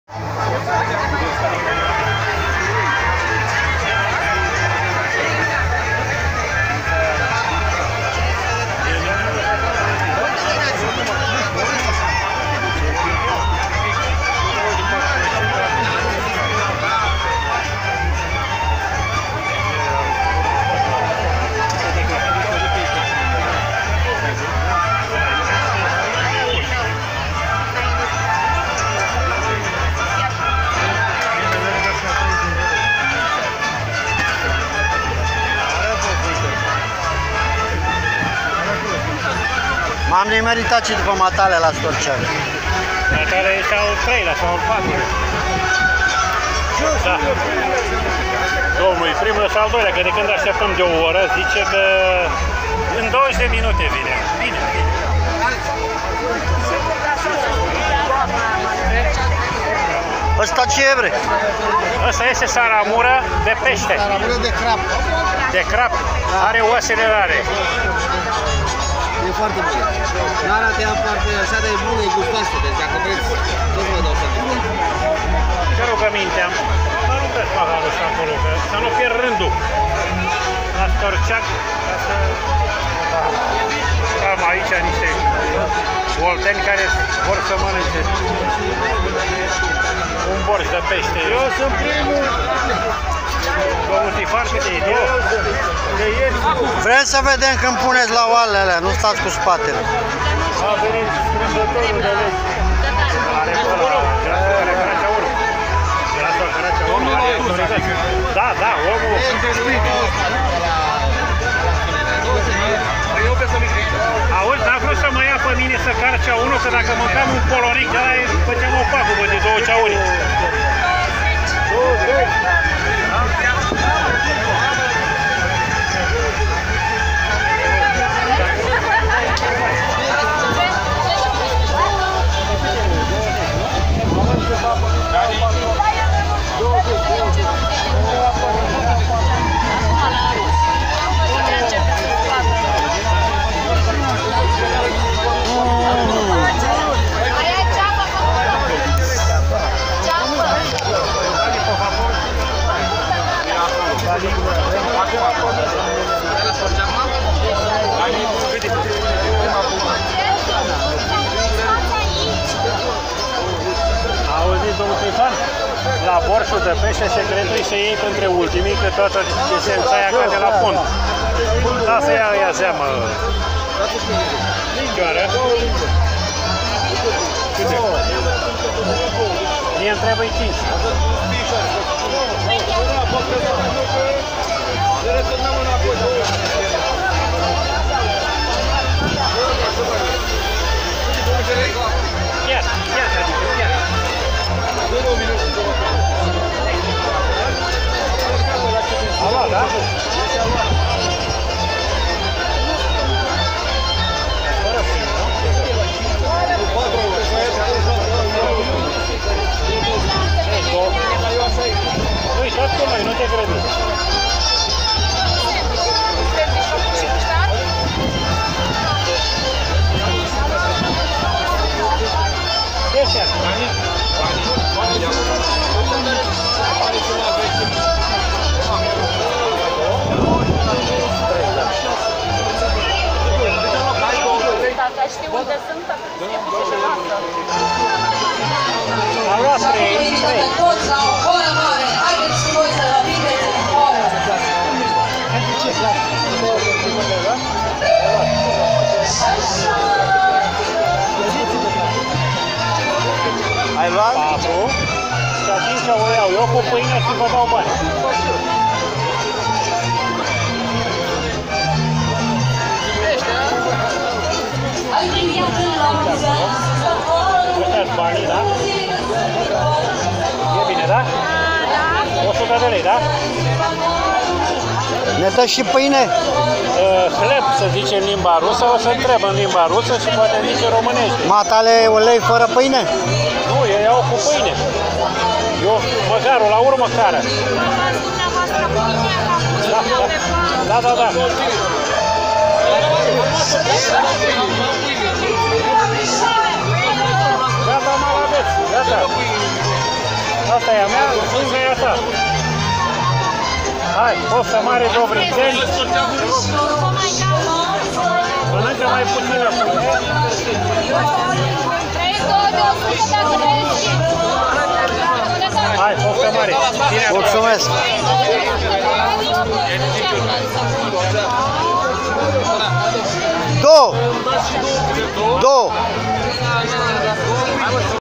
Festivalul borsului de peste Crisan ,sambata(2)
Arome ,prajituri de casa ( de o iei de nebun) peste in toate felurile posibile .